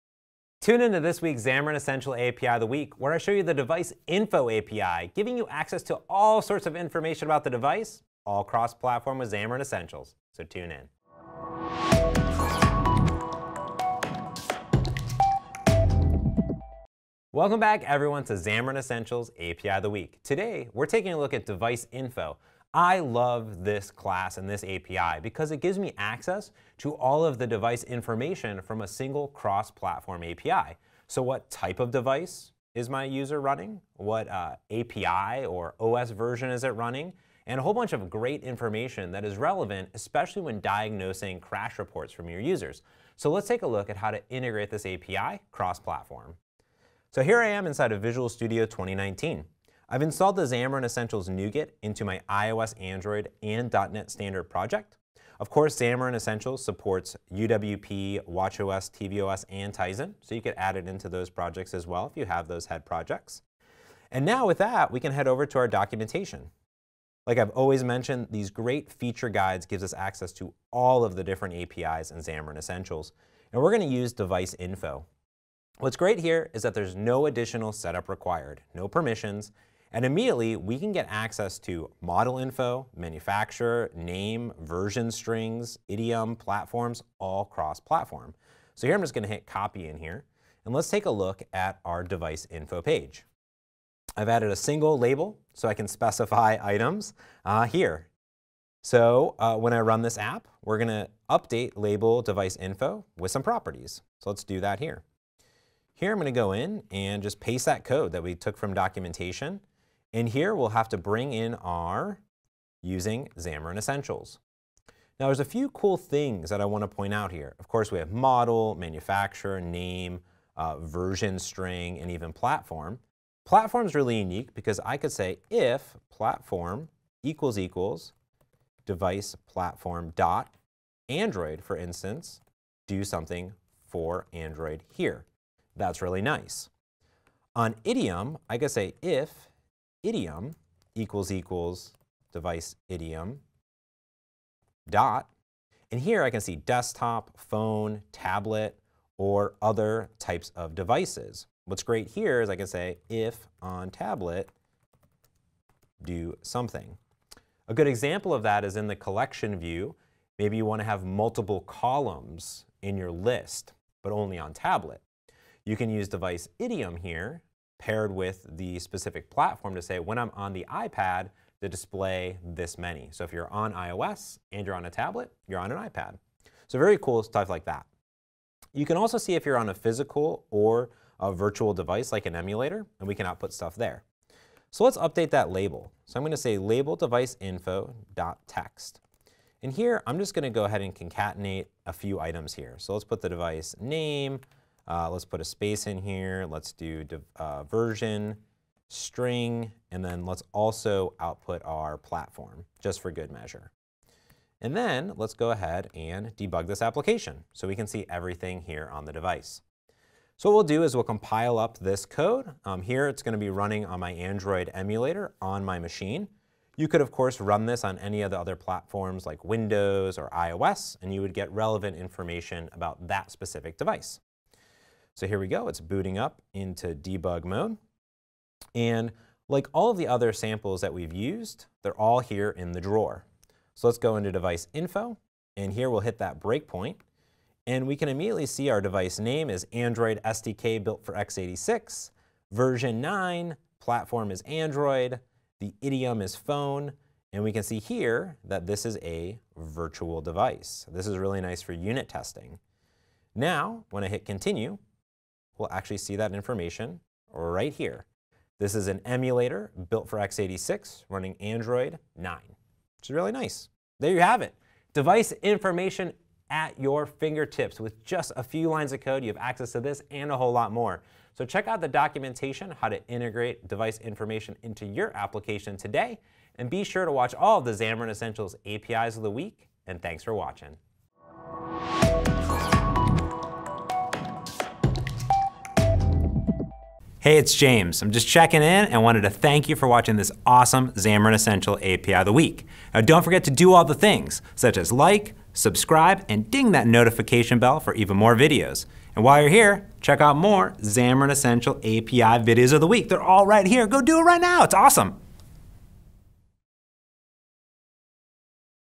Device Information (Xamarin.Essentials API of the Week)
Xamarin.Essentials provides developers with cross-platform APIs for their mobile applications. On this week's Xamarin.Essential API of the week we take a look at the Device Info API that can give developers information about the device they are using.